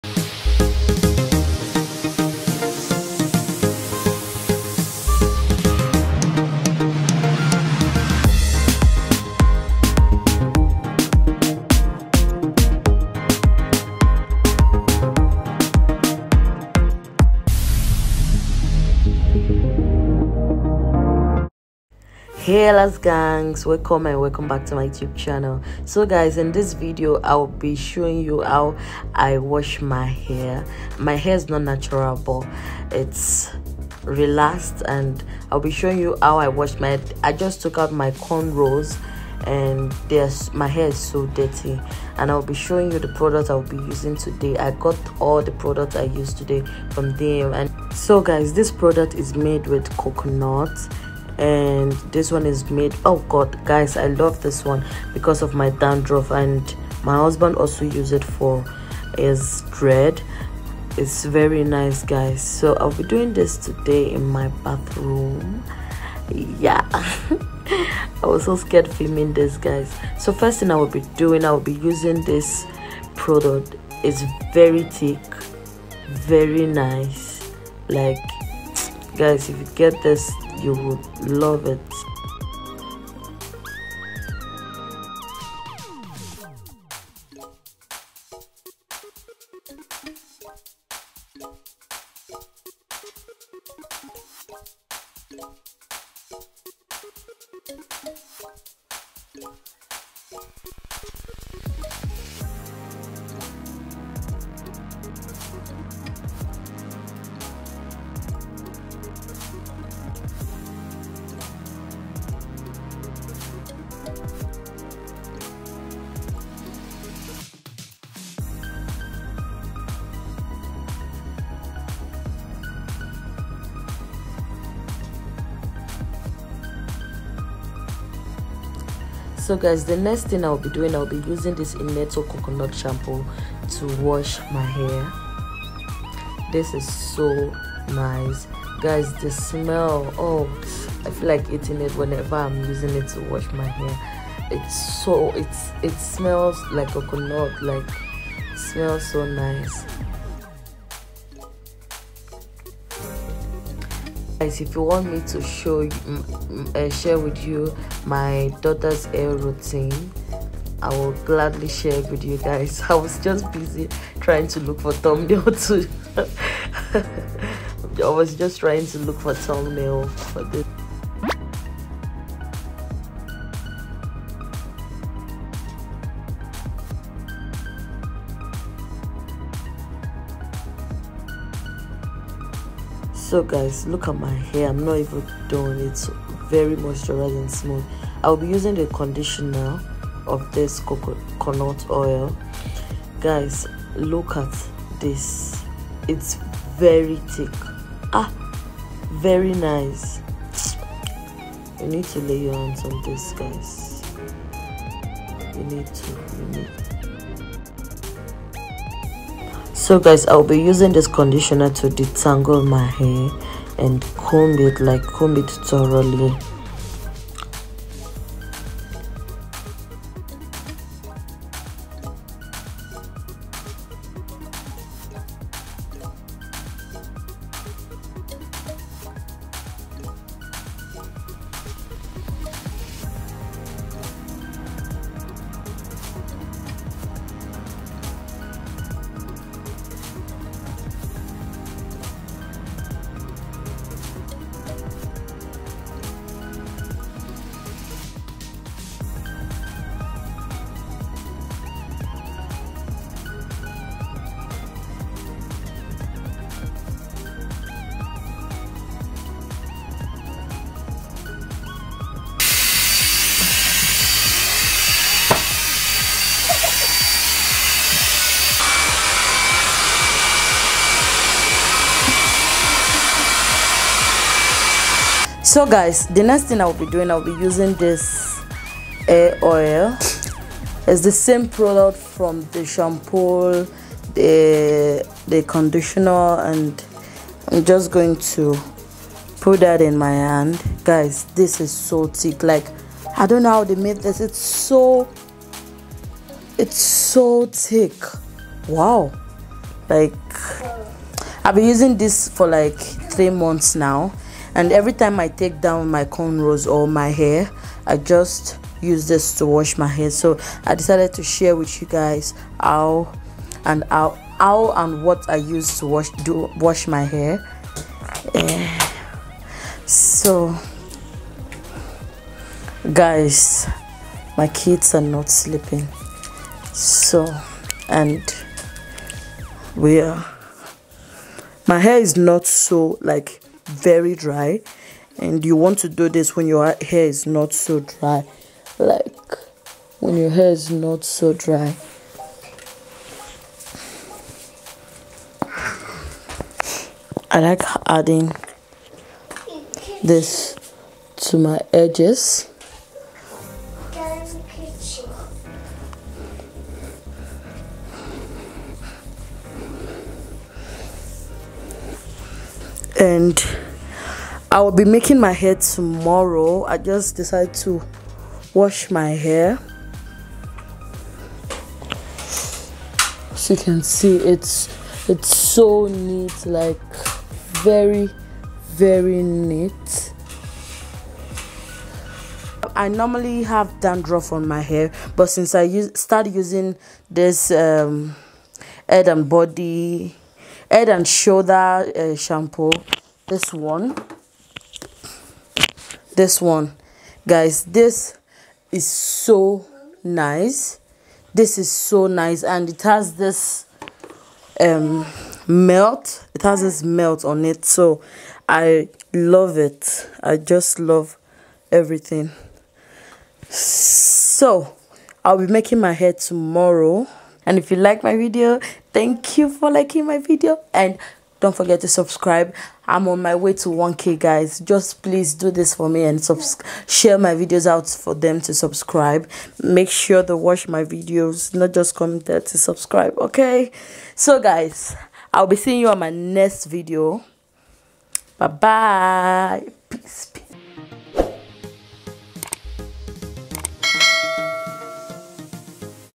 MY BASIC AND AFFORDABLE WASH DAY ROUTINE (start to finish)
Hey love

today I am going sharing with you my basic and affordable wash day regimen learn how to wash
Your relax hair

How to do simple makeup but glam